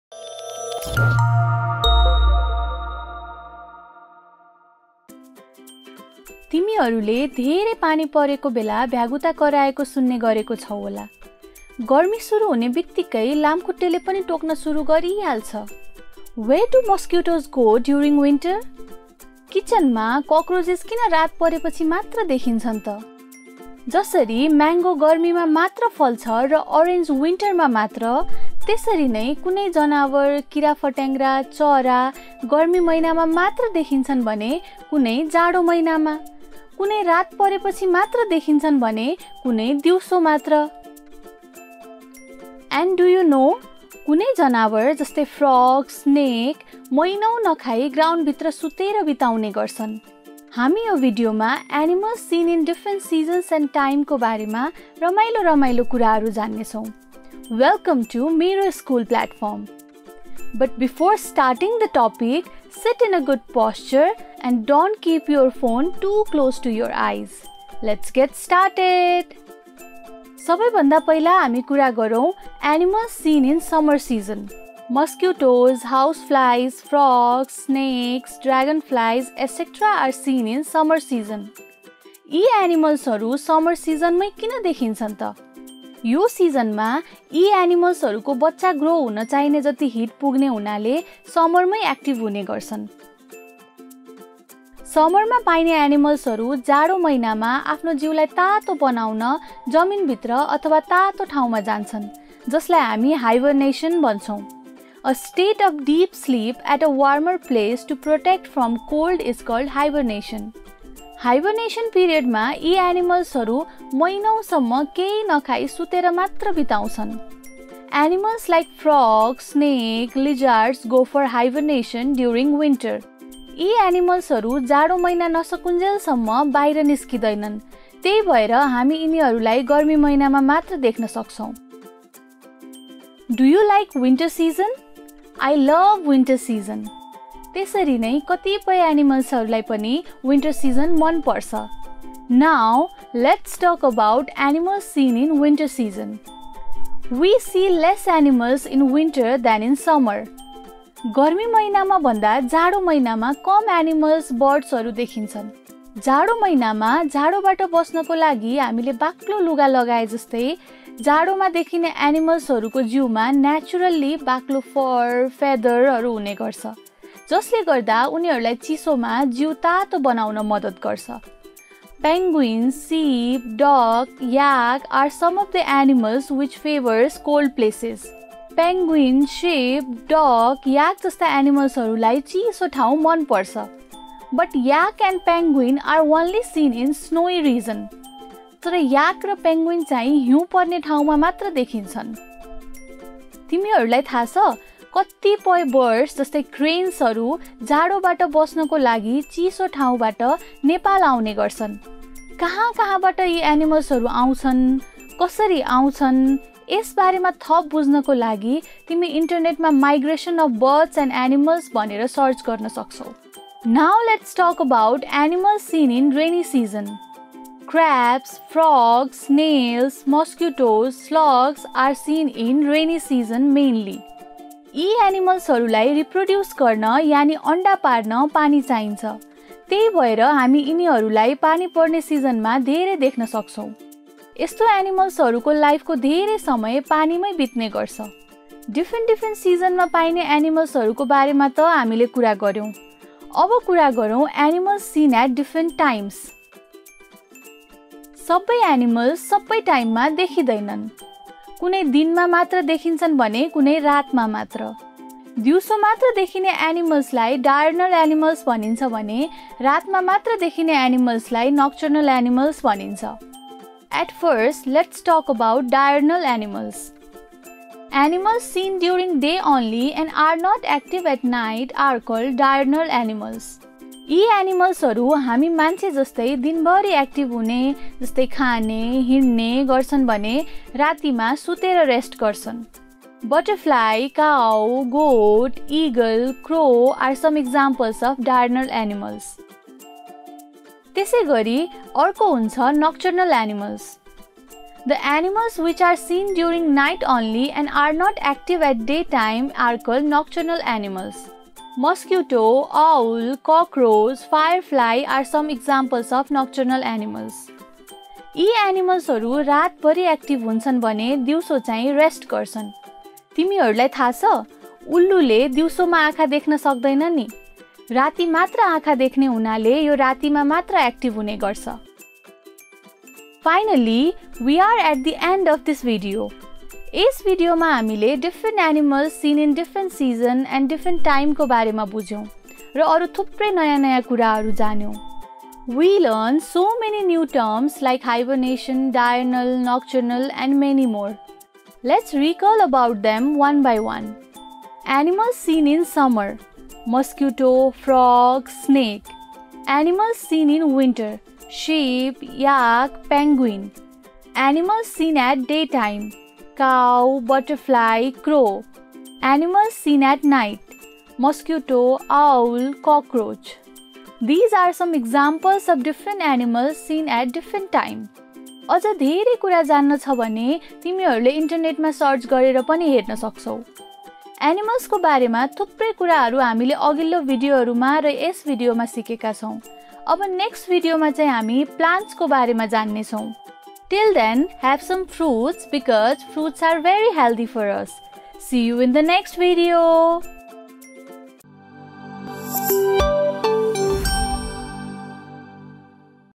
Animals Seen In Different Seasons & Time | Grade 3 | Science | Mero School Nepal
Animals Seen In Different Seasons and Time
Video Lesson on Animals Seen In Different Seasons & Time from Grade 3 Science By Mero School based on Nepal's Education Board's Curriculum.